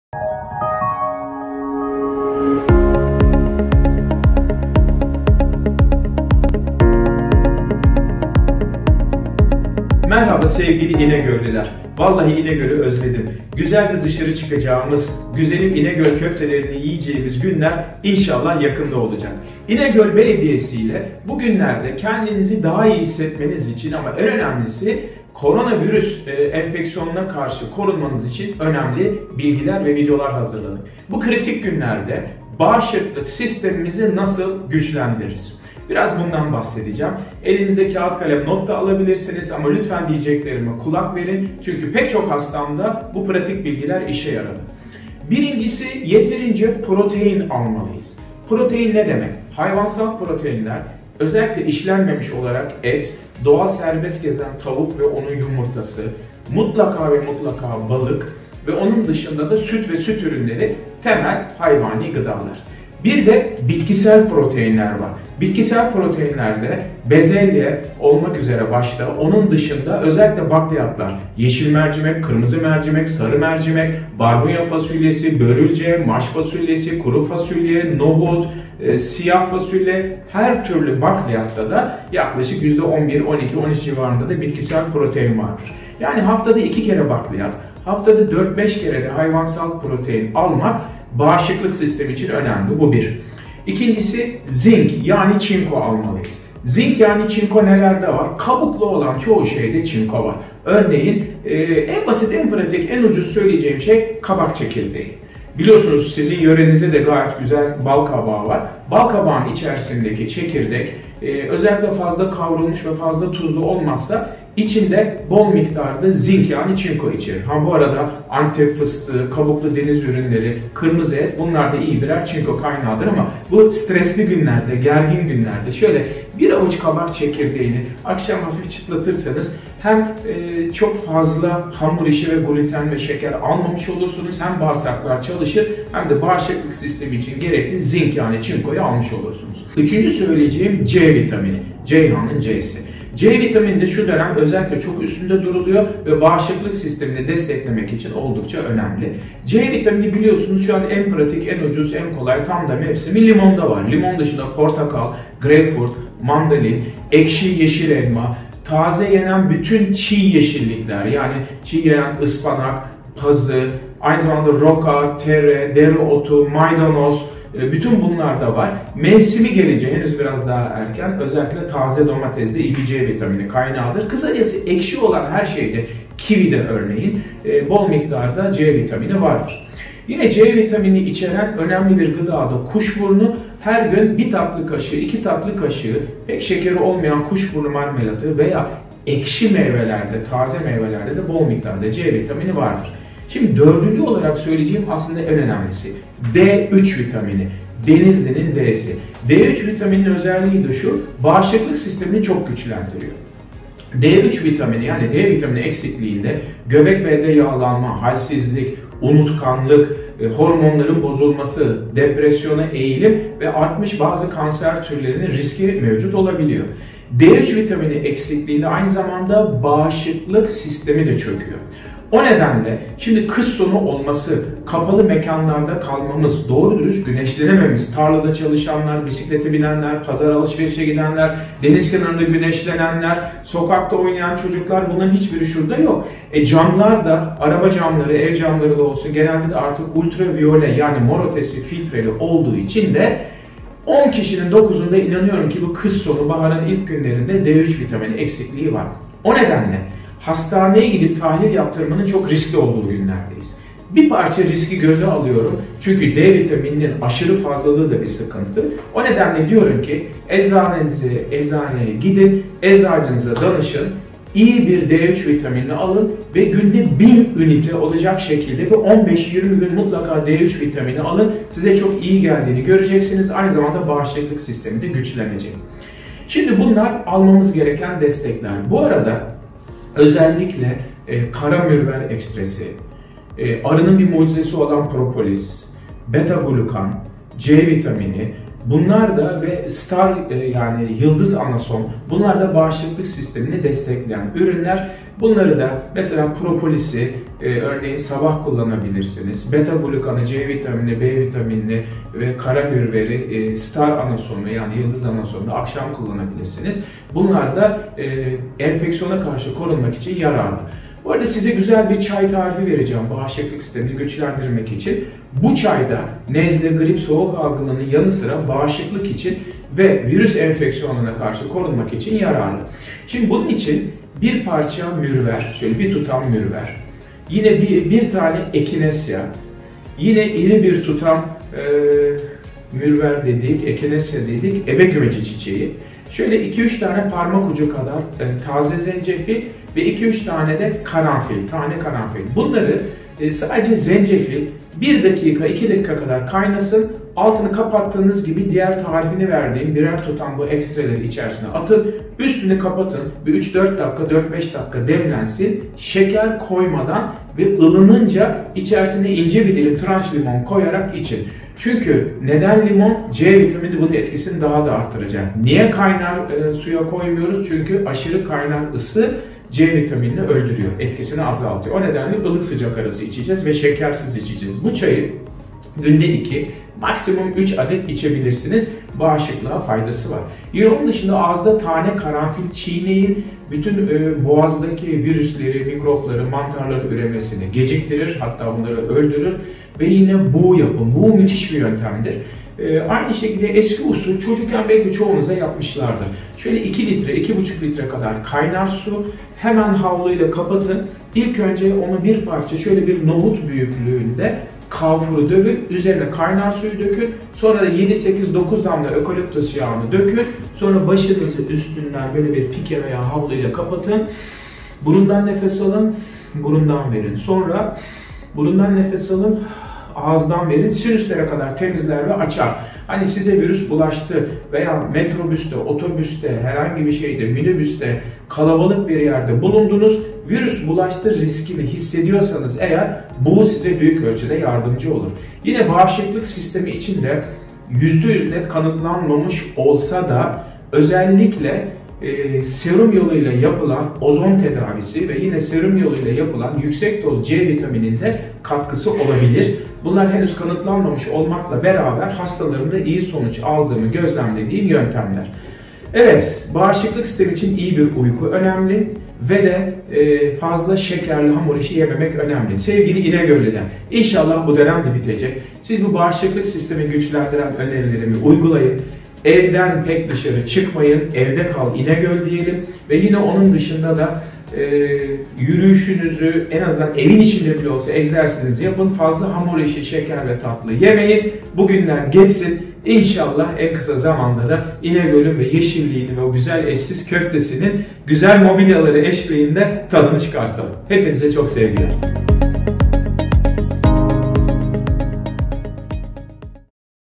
Dr. Ender Saraç ile Sağlığa Dair / 2. Bölüm
Siz sağlıkla evde kalın, biz size geliriz.
Evde kaldığımız bu günlerde bağışıklık sistemimizi nasıl güçlendiririz ?
Dr. Ender Saraç'tan size tavsiyeler..